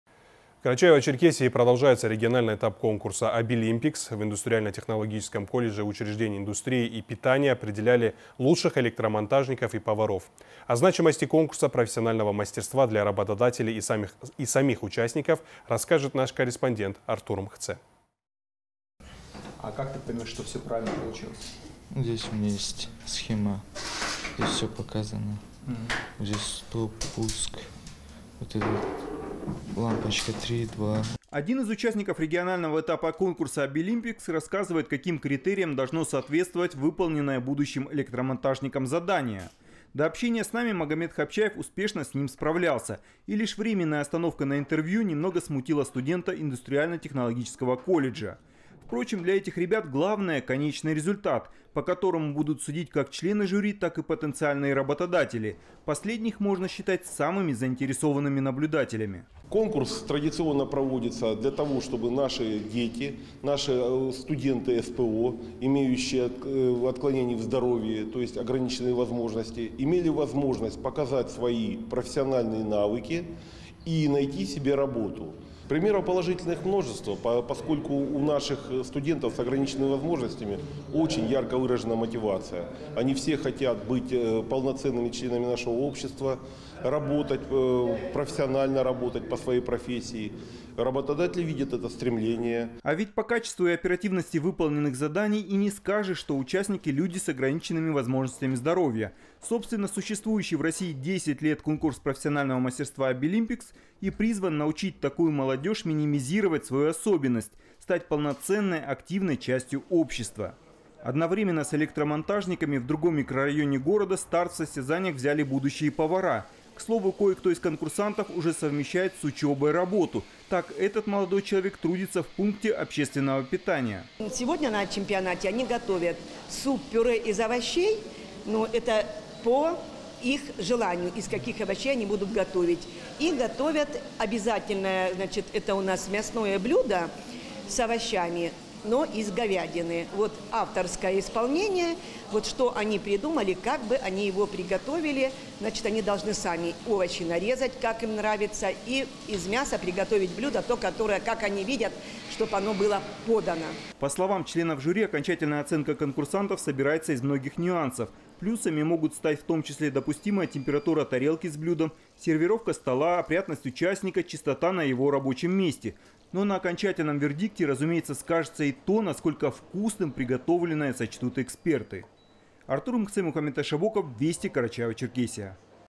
В Карачаево-Черкесии продолжается региональный этап конкурса "Абилимпикс"
В Карачаево-Черкесии продолжается региональный этап конкурса "Абилимпикс". В Индустриально-технологическом колледже учреждения индустрии и питания определяли лучших электромонтажников и поваров.